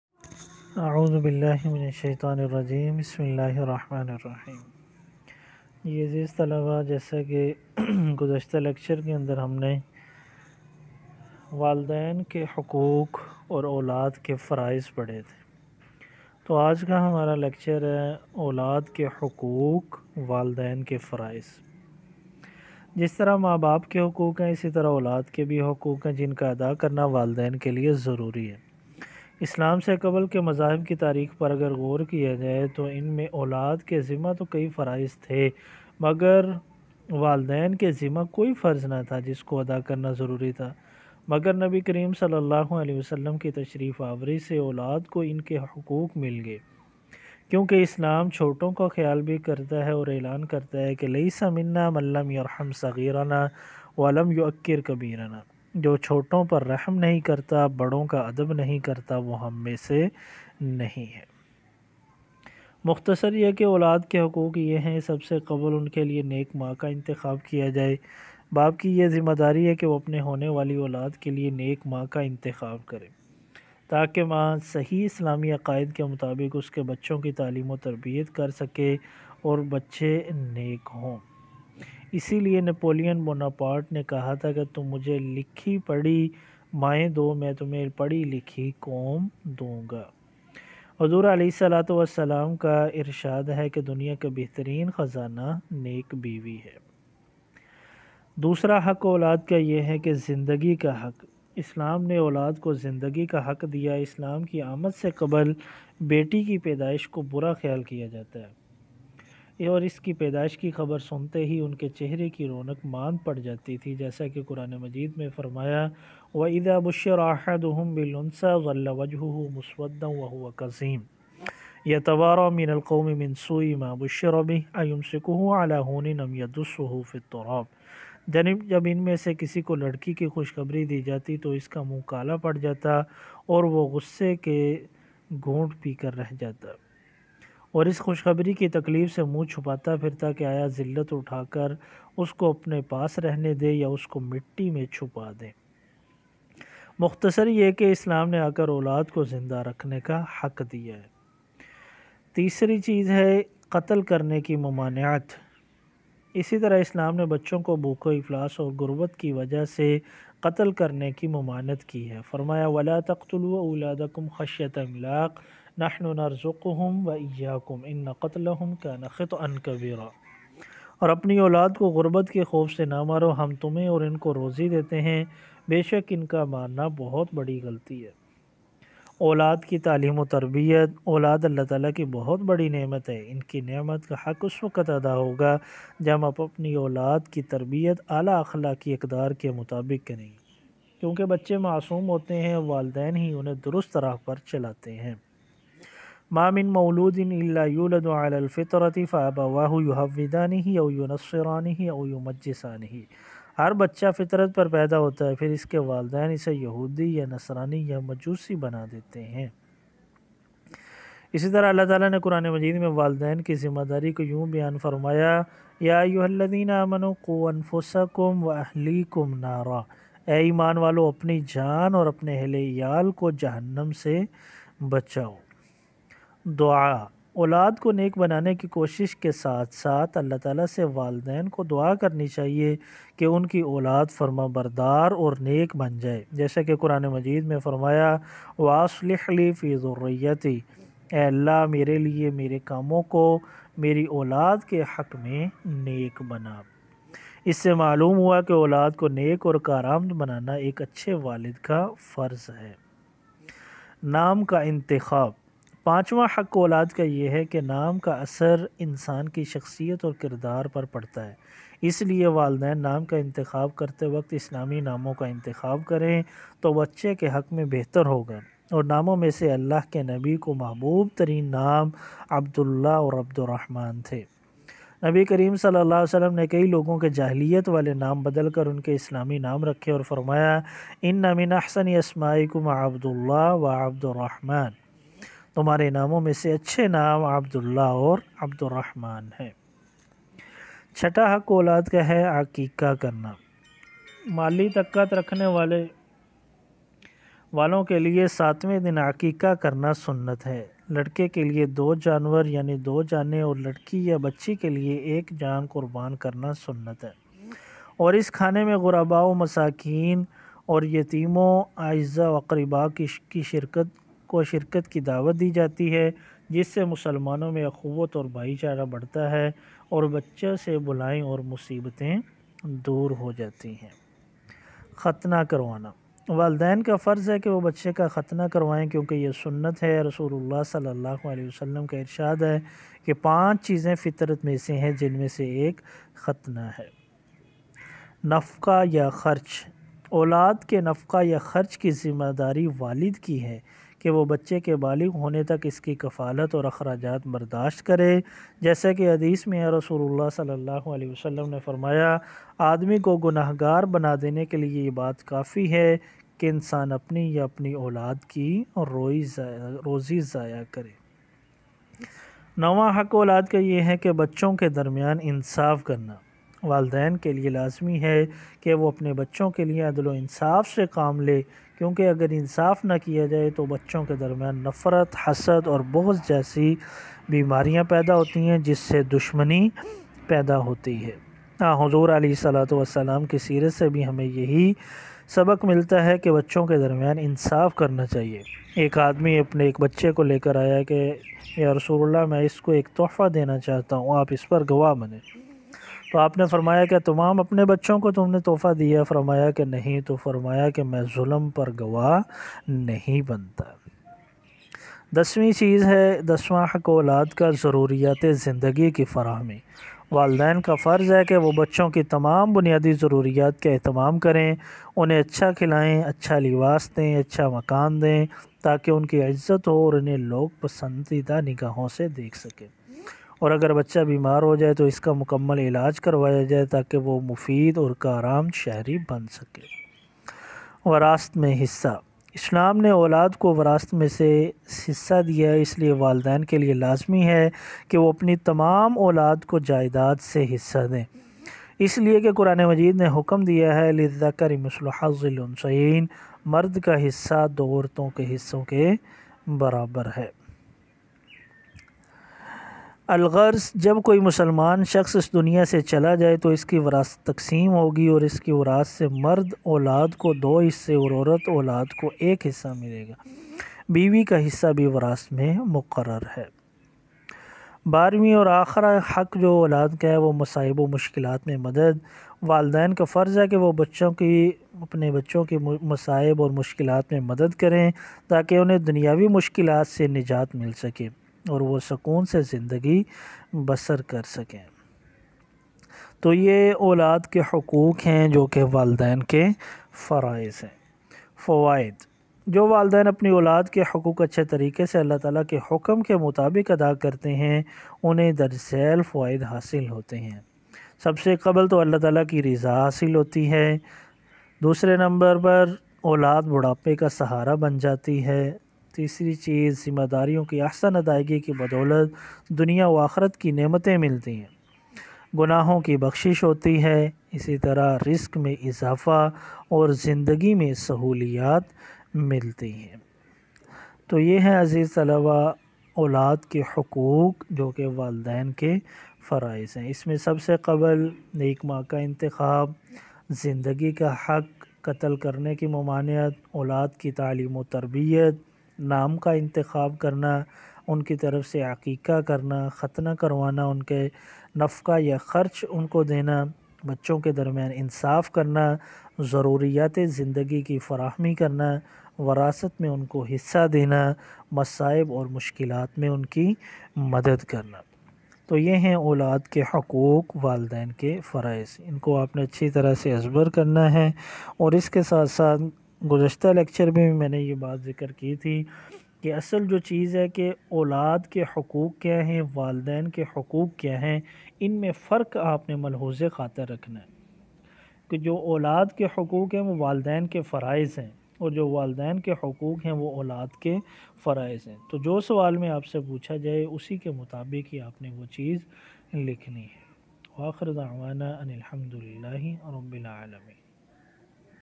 اولاد کے حقوق اور والدین کے فرائض
Lecture by Prof. Hafiz Ihsan Ullah